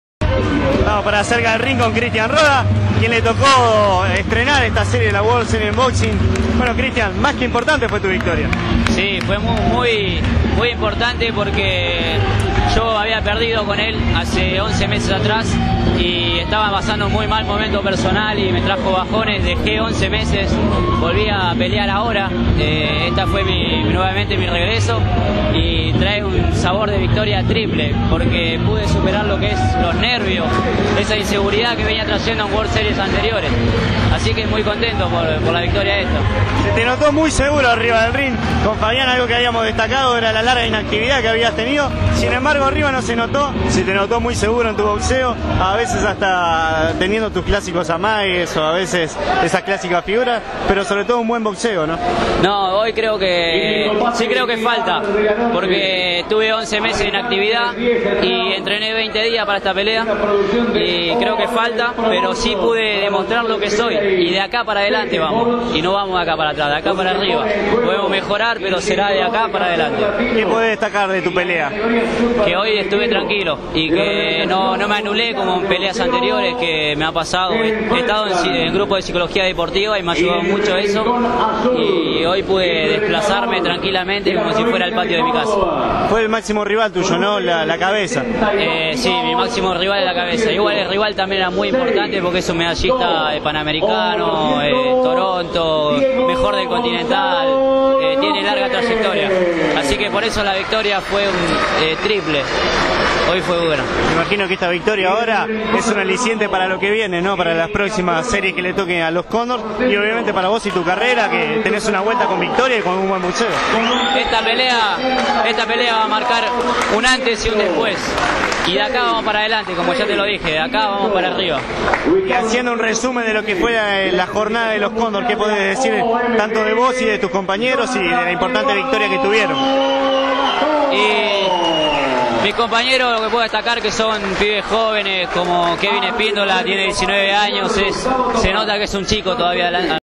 Cristian Roda
Nota realizada a Cristian Roda, boxeador argentino que le toco abrir la  fecha  se impuso por puntos en fallo dividido ante su par Venezolano Yoel Finol, dandole de esta manera el primer triunfo para el combinado de los Condors, pactada en categoría de los 52 kilogramos.